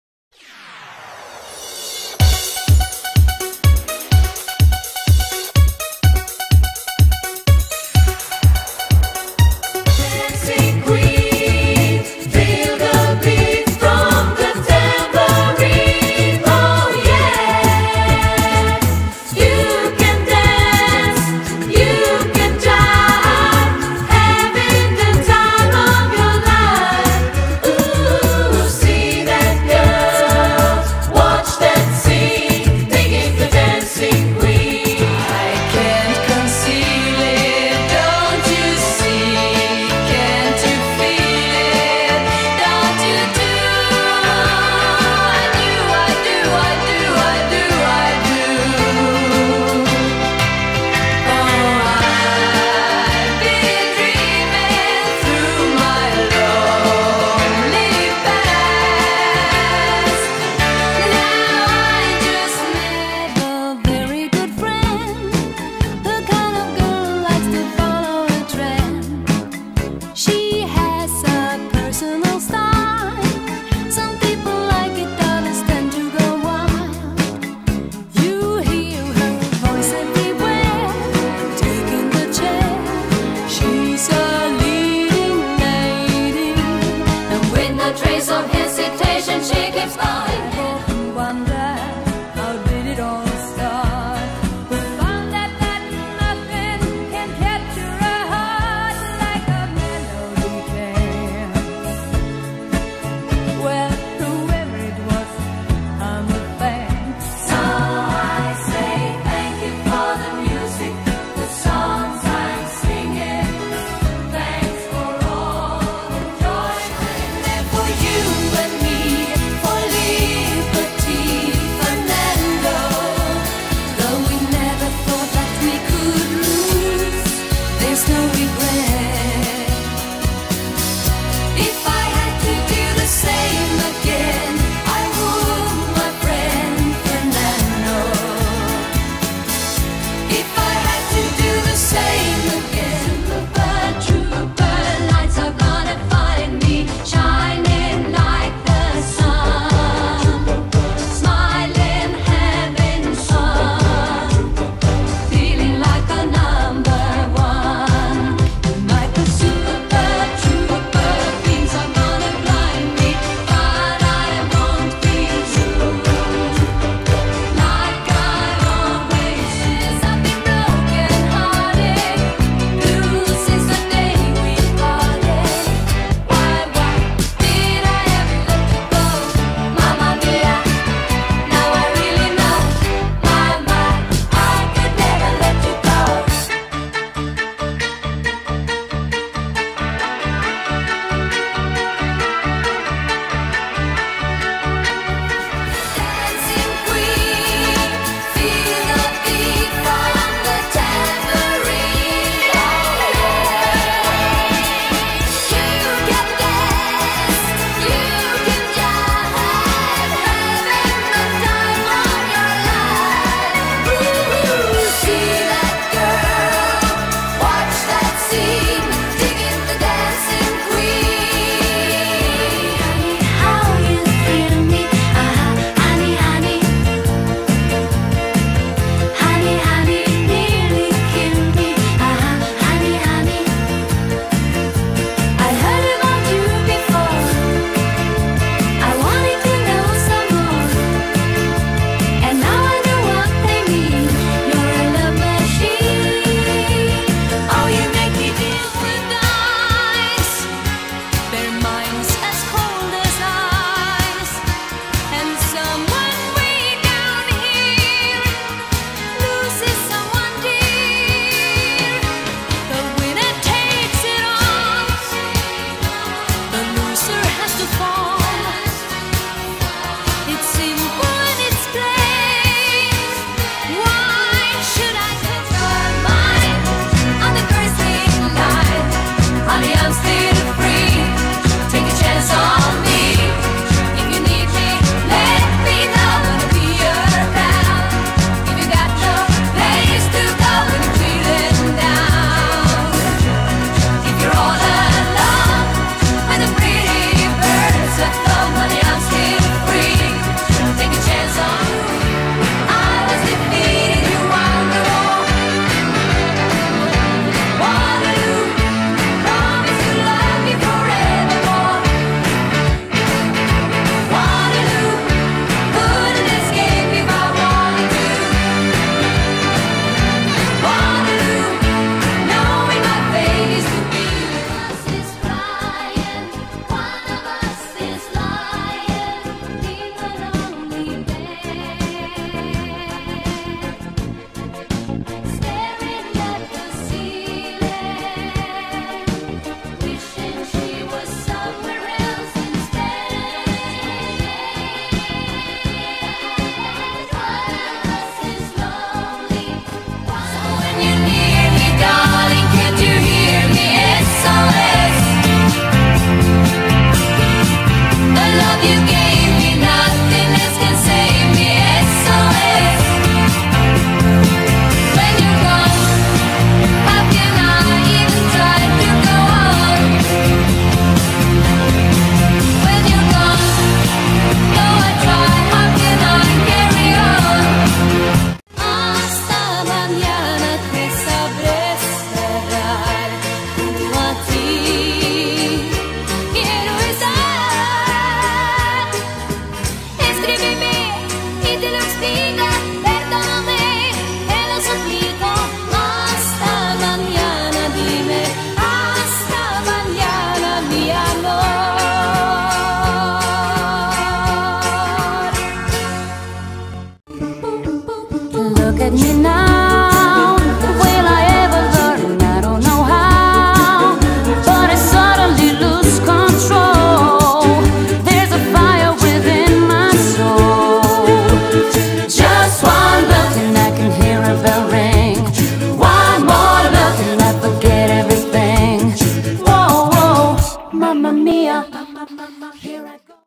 EXITOS ENCADENADOS : ABBA
Recordar éxitos de la música de ayer y siempre con los artistas que hicieron historia en la música nacional e internacional.
Entrega nº1 : ABBA .
Próxima entrega : LOS BEATLES